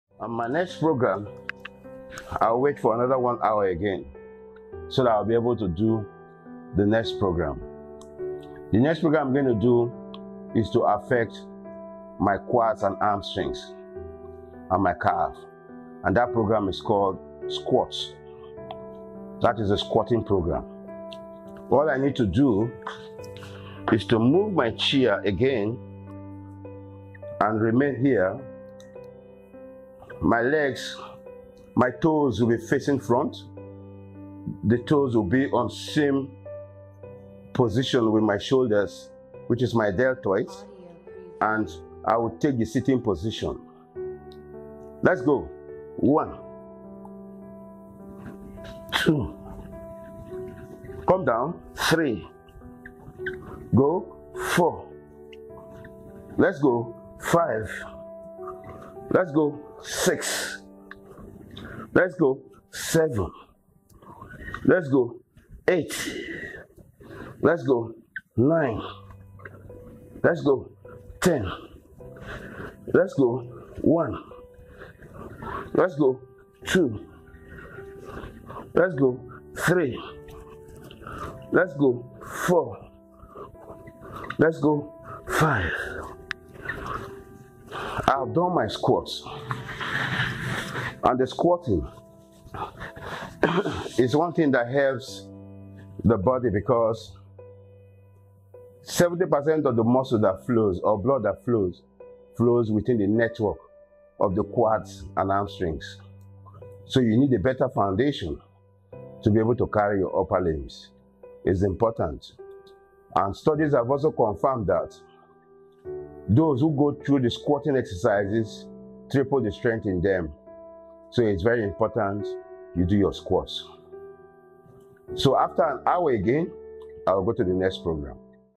Power by the hour quads
Exercise regiment for your quads and Hamstring while in the office. An excerpt of Power by the hour by Soye Elekima